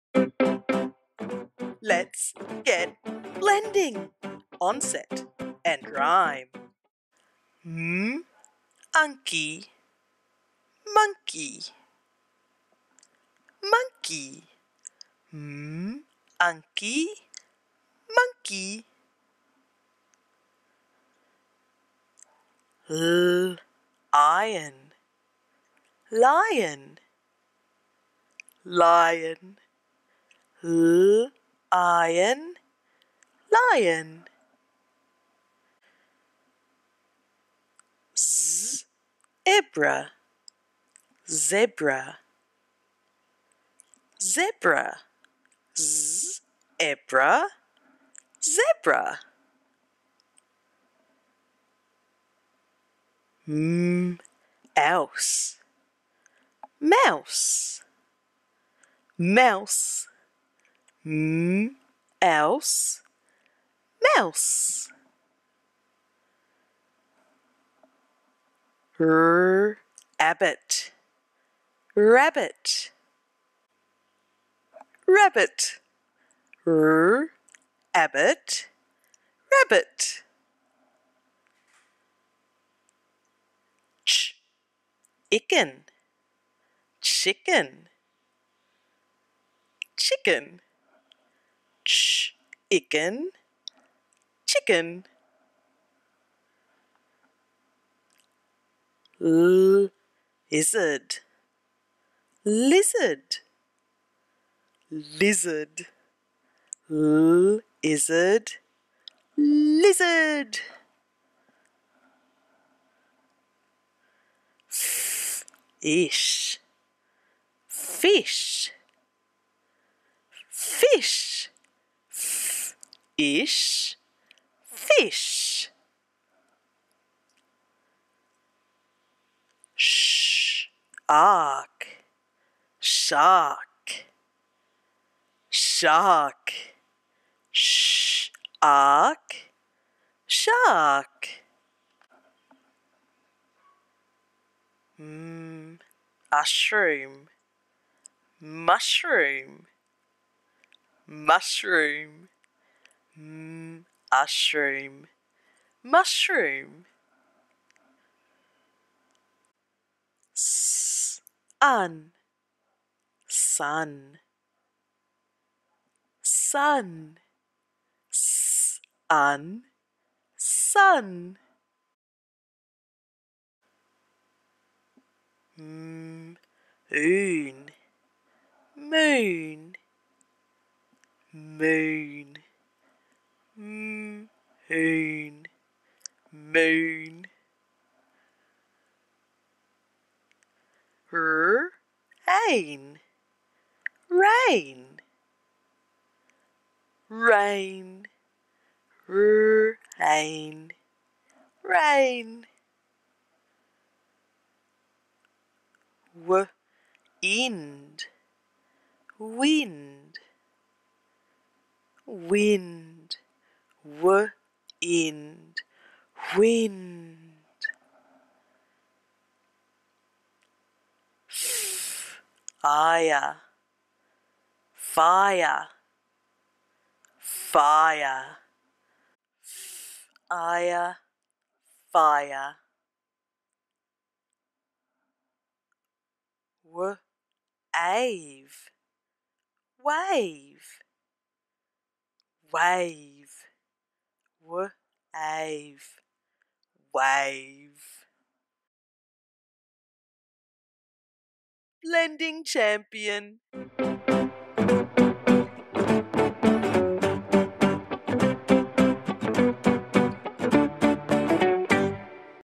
Phonological Awareness - Blending Onset and Rime - Australian Accent - Animals and Nature
Onset and rime blending with Emojis!

Great practice of phonological awareness Onset and rime blending skills.

Kids can just listen, listen and repeat, or try blend the sounds before they hear the answer.

Being able to blend sounds in words quickly and automatically is a critical early reading skill. Kids need to be able to do this to crack the code of the alphabet and learn to decode.

These videos are great for kids who are learning how to blend sounds, or who need extra practice consolidating the skills.

In these videos the words are blending slowly and explicitly before and after seeing a picture. This helps kids get all the practise they need, without being overwhelmed by unnecessary information.